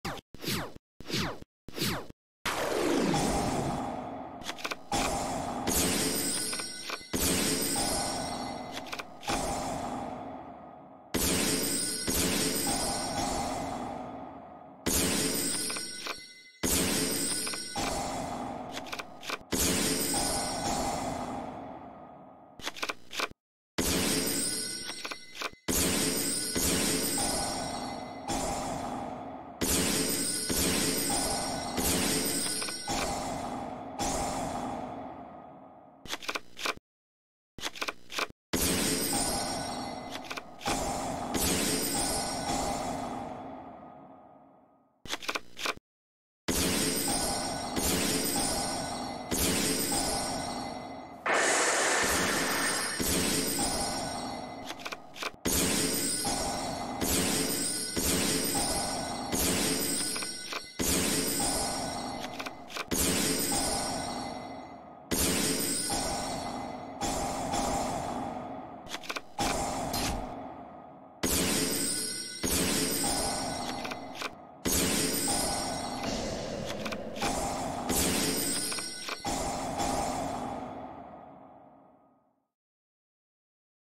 1 Card Mekk Knight Full Hand and Extra Deck Destruction TCG/OCG Post Duel Overload/Eternity Code
Send their entire Hand and Extra Deck to the Graveyard with the Mekk-Knight Field Spell.